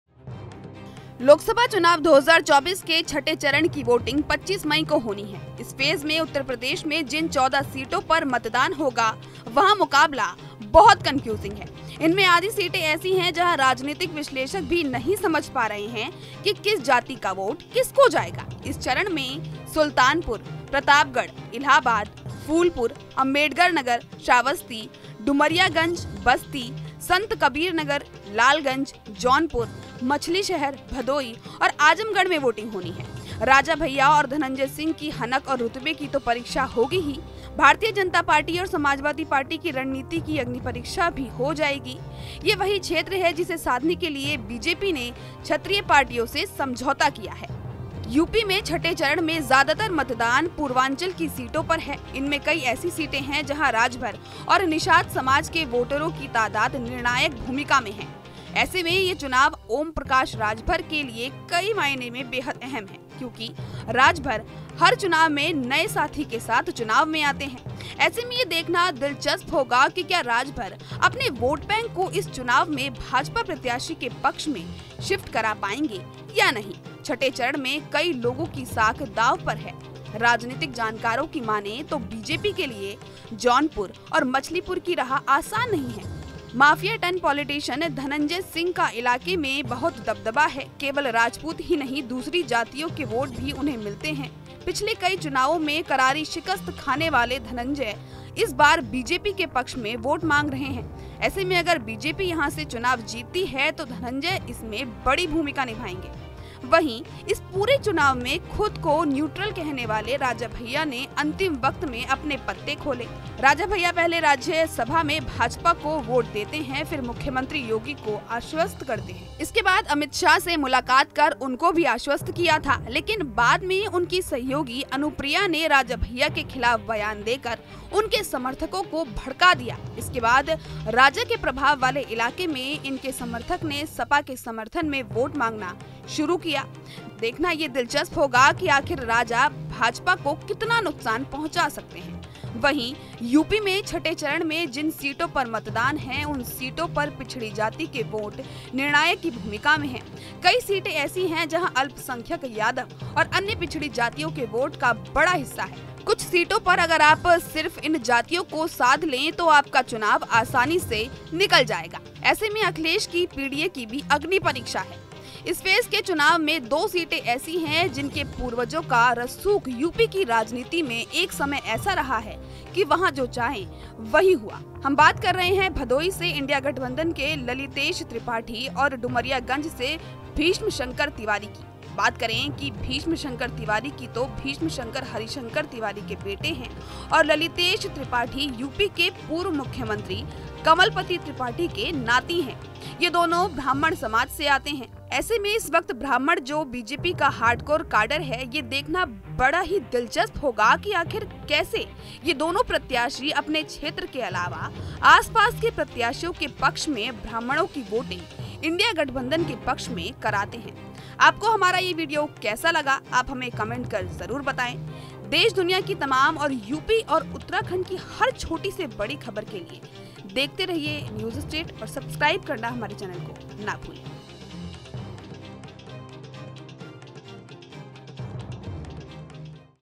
Sixth Phase Voting: पूर्वांचल की इन 14 सीटों पर दांव पर है SP-BJP की प्रतिष्ठा | Akhilesh Yadav
लोकसभा चुनाव 2024 के छठे चरण की वोटिंग 25 मई को होनी है. इस फेज में उत्तर प्रदेश में जिन 14 सीटों पर मतदान होगा वहां मुकाबला बहुत कन्फ्यूजिंग है. इनमें आधी सीटें ऐसी हैं जहां राजनीतिक विश्लेषक भी नहीं समझ पा रहे हैं कि किस जाति का वोट किसको जाएगा. इस चरण में सुल्तानपुर, प्रतापगढ़, इलाहाबाद, फूलपुर, अंबेडकरनगर, श्रावस्ती, डुमरियागंज, बस्ती, संतकबीरनगर, लालगंज, जौनपुर,मछलीशहर, भदोही और आजमगढ़ में वोटिंग होनी है.

News State Apps डाउनलोड करें:
Download Android and iOS Apps for more updates: